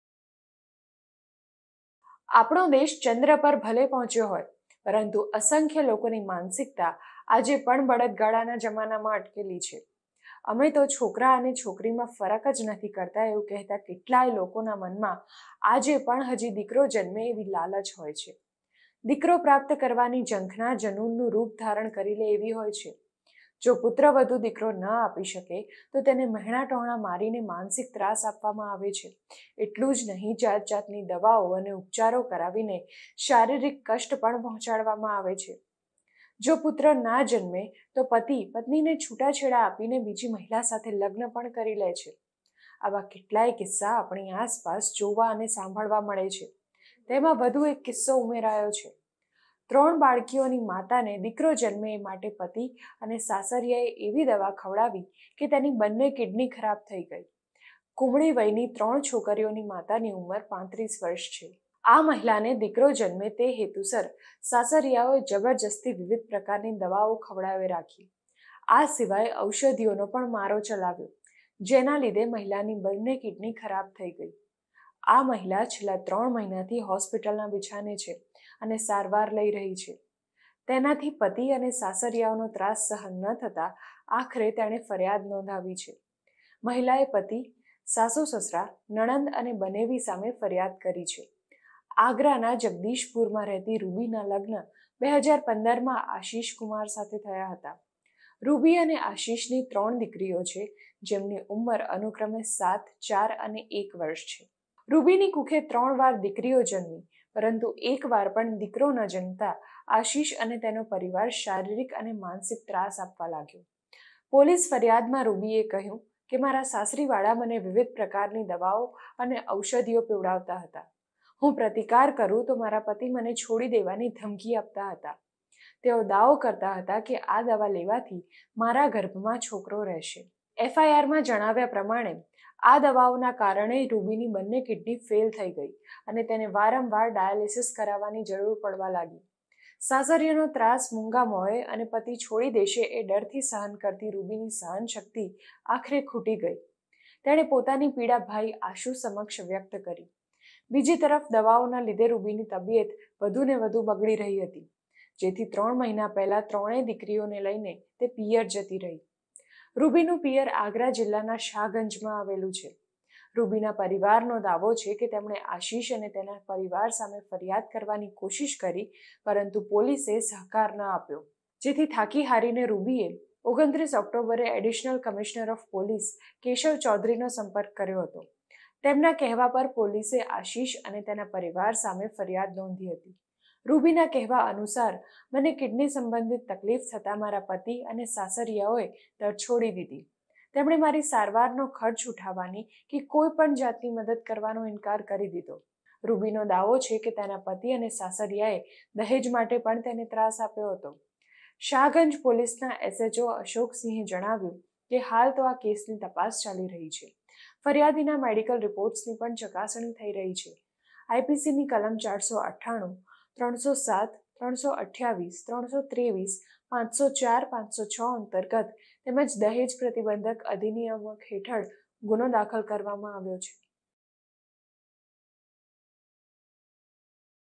સાસરિયાઓનો પુત્ર મોહ વહુની કિડની 'ભરખી ગયો'
આપણો દેશ ચંદ્ર પર ભલે પહોંચ્યો હોય પરંતુ અસંખ્ય લોકોની માનસિકતા આજે પણ બળદગાડાના જમાનામાં અટકેલી છે. અમે તો છોકરા અને છોકરીમાં ફરક જ નથી કરતાં કેટલાય લોકોના મનમાં આજે પણ હજી દીકરો જન્મ તેવી લાલચ હોય છે. દીકરો પ્રાપ્ત કરવાની ઝંખના ઝનૂનનું રૂપ ધારણ કરી લે એવી હોય છે. જો પુત્રવધૂ દીકરો ના આપી શકે તો તેને મહેણાં-ટોણાં મારીને માનસિક ત્રાસ આપવામાં આવે છે. ઉપરાંત જાતજાતની દવાઓ અને ઉપચારો કરાવીને શારીરિક કષ્ટ પણ પહોંચાડવામાં આવે છે. જો પુત્ર ના જન્મ તો પતિ છૂટાછેડા આપીને બીજી મહિલા સાથે લગ્ન પણ કરી લે છે. આવા કેટલાય કિસ્સા આપણી આસપાસ જોવા-સાંભળવા મળે છે. તેમાં વધુ એક કિસ્સાનો ઉમેરો થયો છે. ત્રણ બાળકીઓની માતાને દીકરો જન્મ એ માટે પતિ અને સાસરિયાએ એવી દવા ખવડાવી કે તેની કિડની ખરાબ થઈ ગઈ. કૂમળી વયની ત્રણ છોકરીઓની માતાની ઉંમર 35 વર્ષ છે.